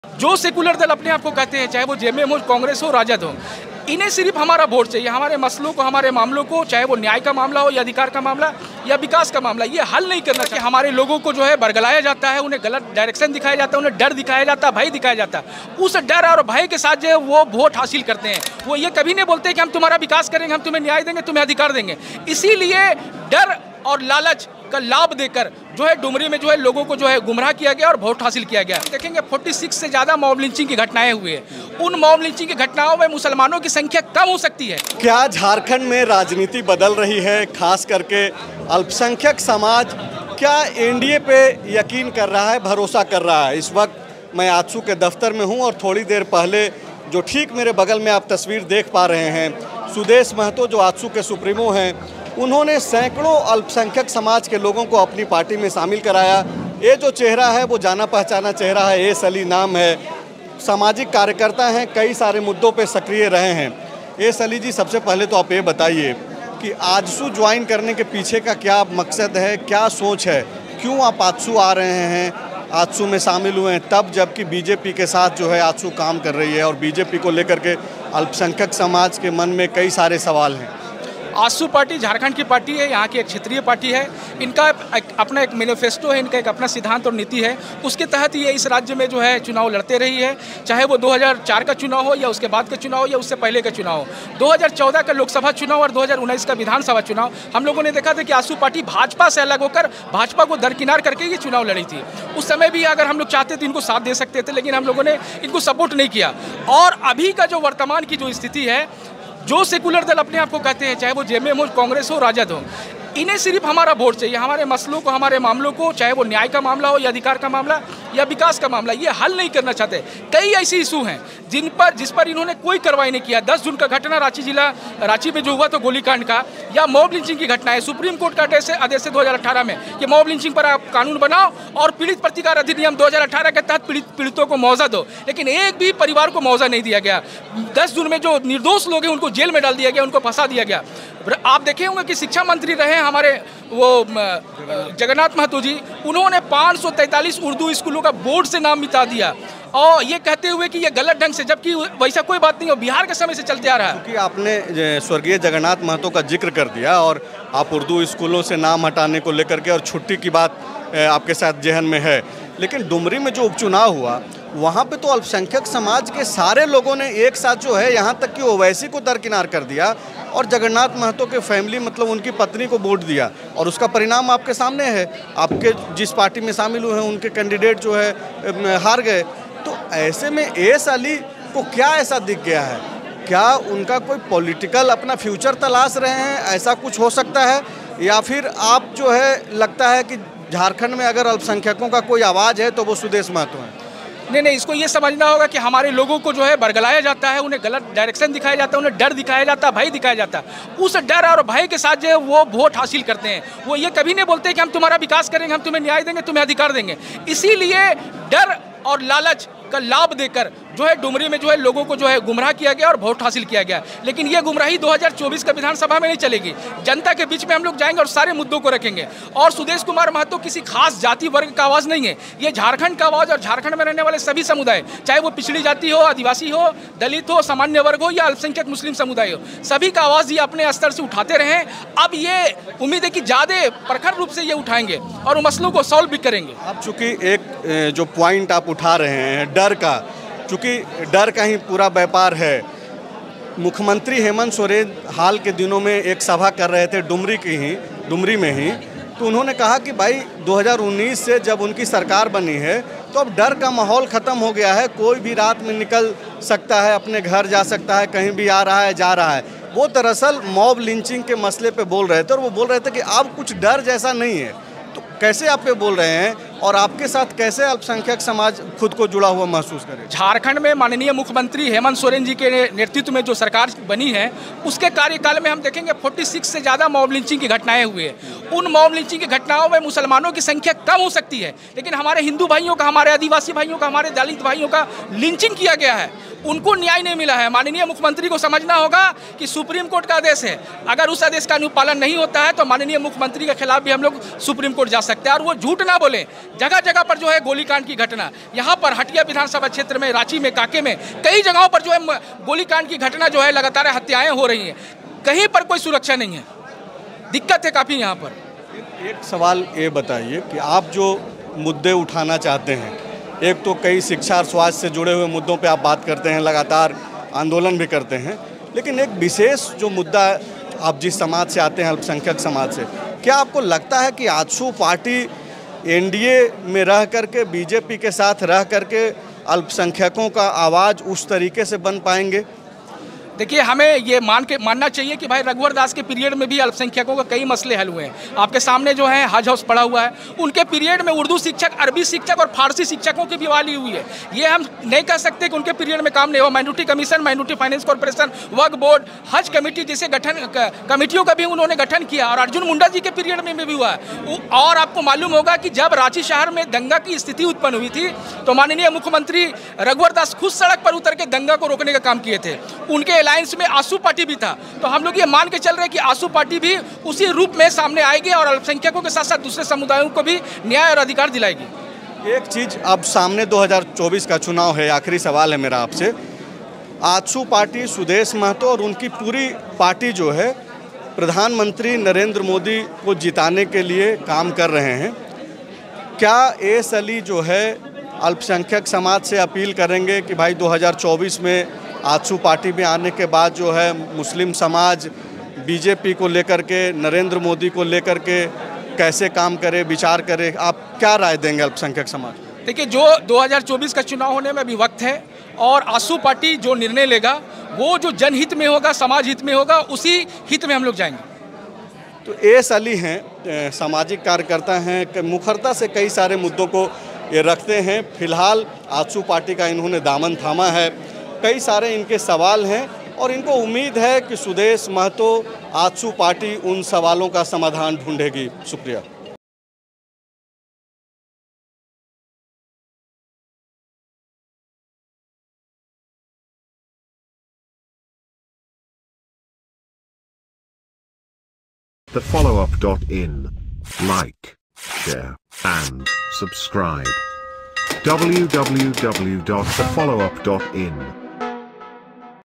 AJSU के हुए S Ali, बोले- Sudesh Mahto हैं झारखंड में मुसलमानों की आवाज
आजसू के हुए एस अली, बोले- सुदेश महतो हैं झारखंड में मुसलमानों की आवाज